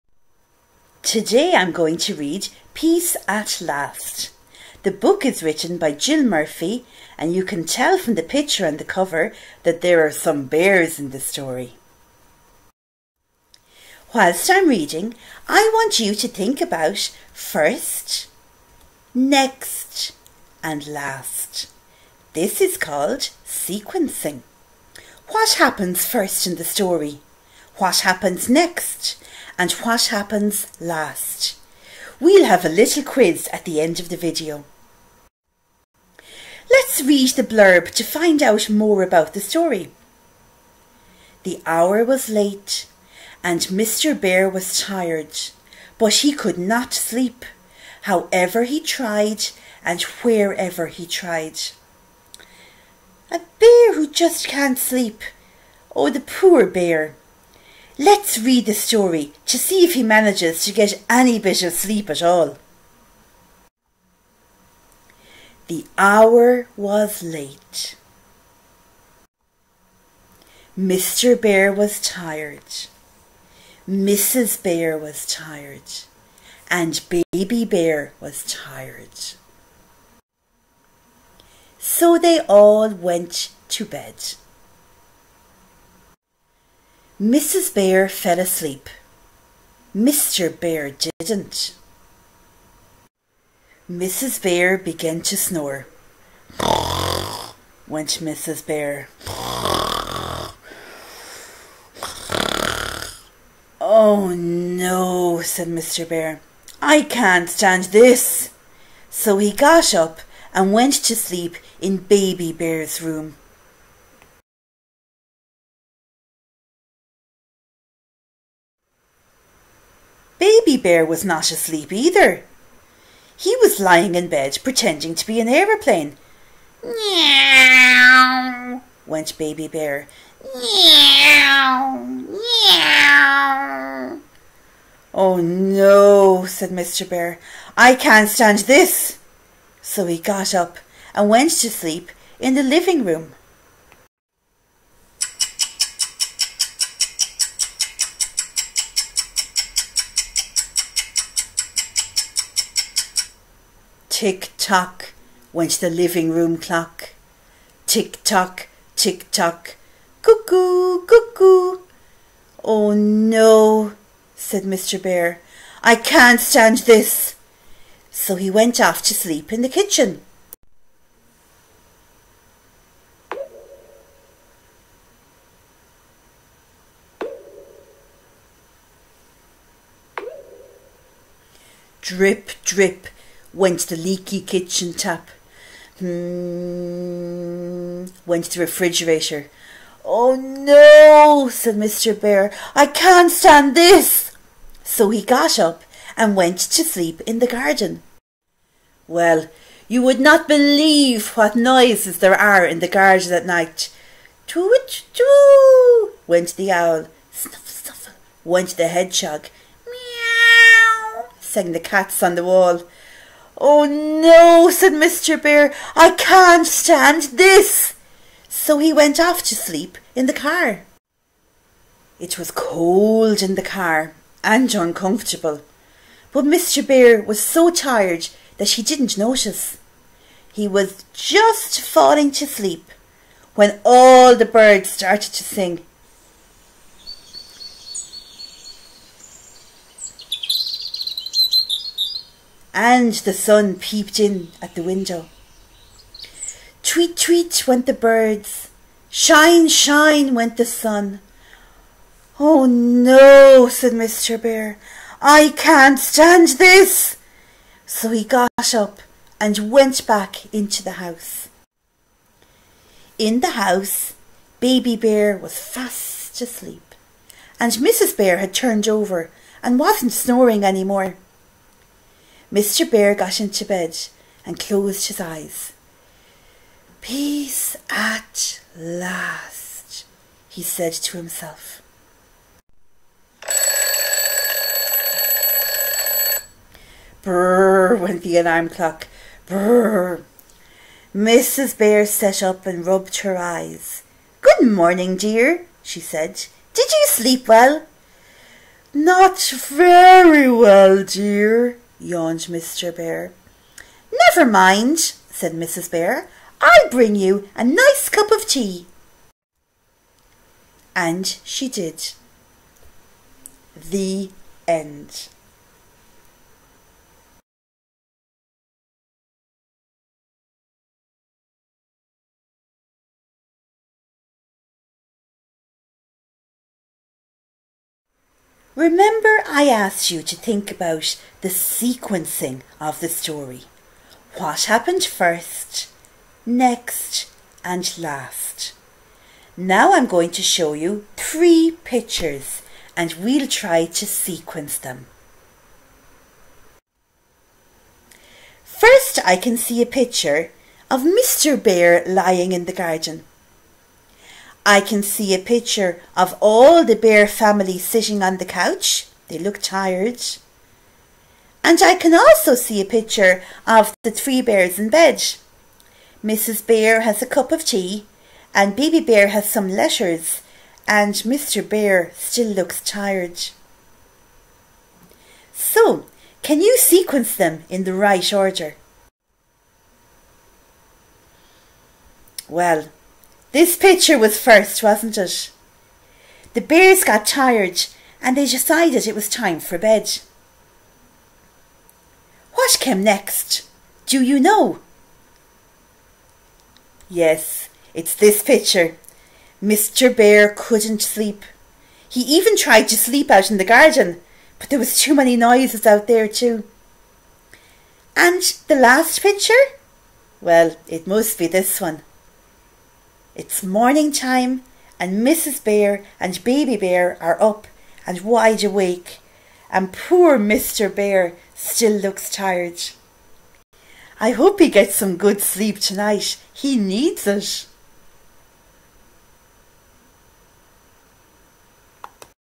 "Peace at Last", read by Mrs O'Meara
This video focuses on Sequencing the story - what comes first, next and last.

Author: Jill Murphy
Publisher: MacMillan Children's Books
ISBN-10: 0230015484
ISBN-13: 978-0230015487